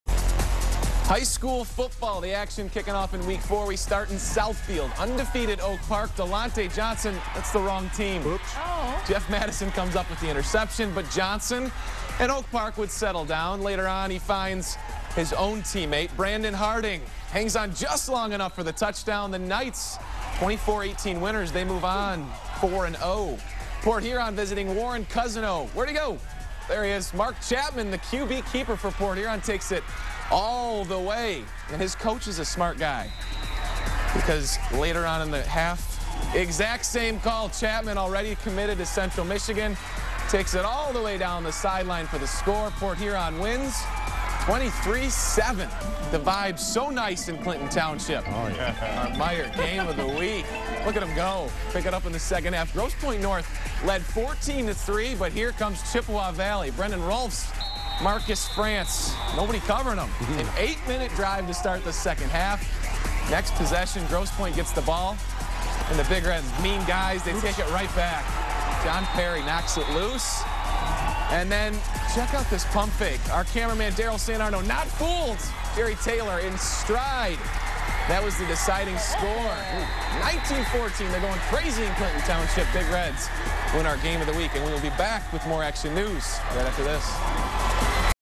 Chippewa Valley wins Meijer Game of the Week
Brad Galli guides you through Chippewa Valley's win in the Meijer Game of the Week, as well as Port Huron's victory over Warren Cousino and Oak Park's win against Southfield.